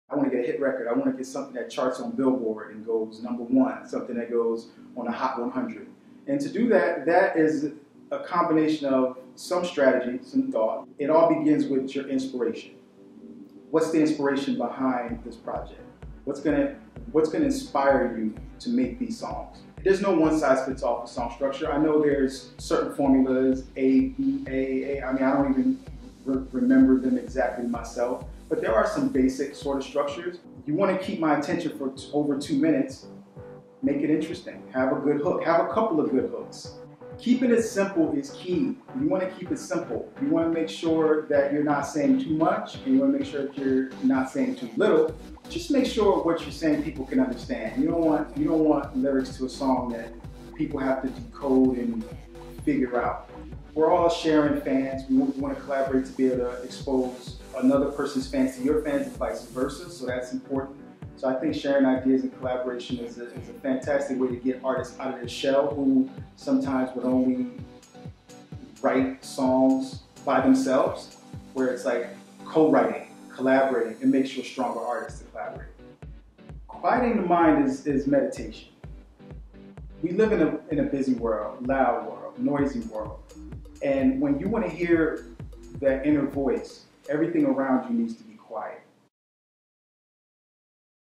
Roy Hamilton III: How To Be Successful in the Music Business
In this video Roy talk about the art of record deals, songwriting, publishing, and placing your music to movies, films and TV shows.